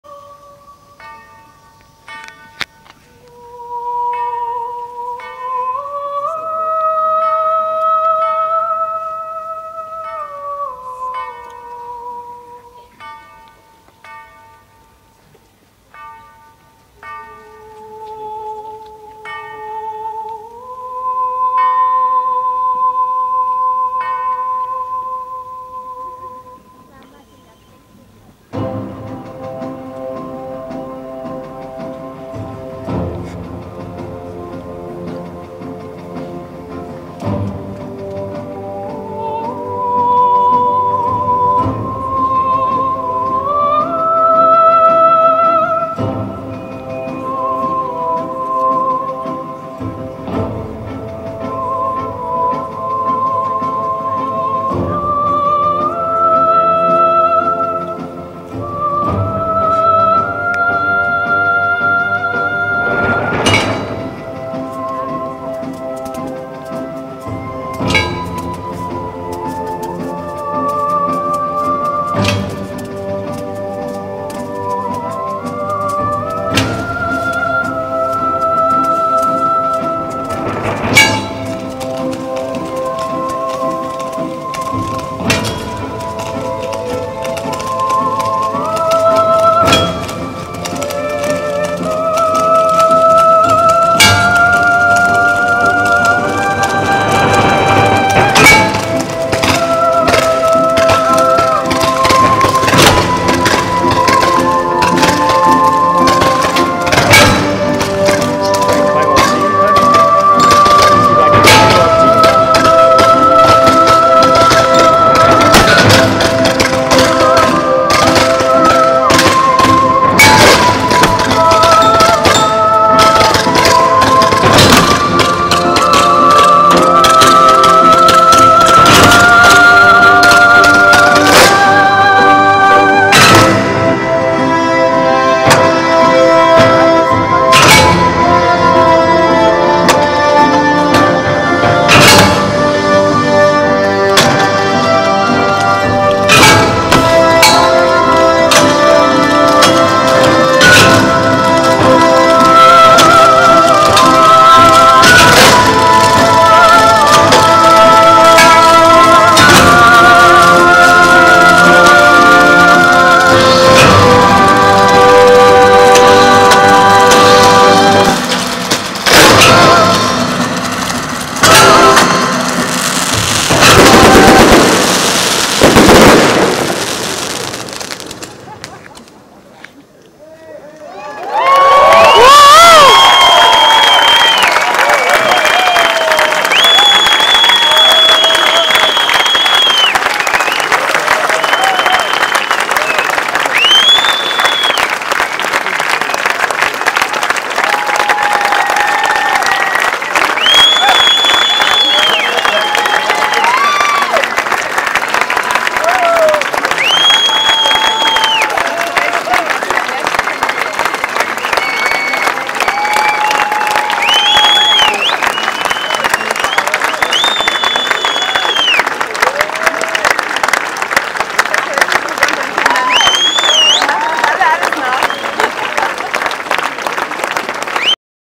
Titanick live HochofenSinfonie 24.9.2010 Leipzig, spektakuläres Finale mit Gesang und Feuerwerk 3/3
mit live Gesang , Feuer , Explosionen, Blitzlichtballongsteigen , Feuerwerk und und und ..

Lange Nacht der Wissenschaften am 24. September 2010
Spektakulärer Anfangs- und Endpunkt dieser Nacht des Wissens waren die Auftritte des Theater Titanick um 18.00 und 23.30 Uhr auf dem Gelände der Alten Messe. Mit der Sinfonie mit drei Hochöfen feierte das Theater gewohnt fantasievoll und feuergewaltig an diesem Abend gleichzeitig seinen 20. Geburtstag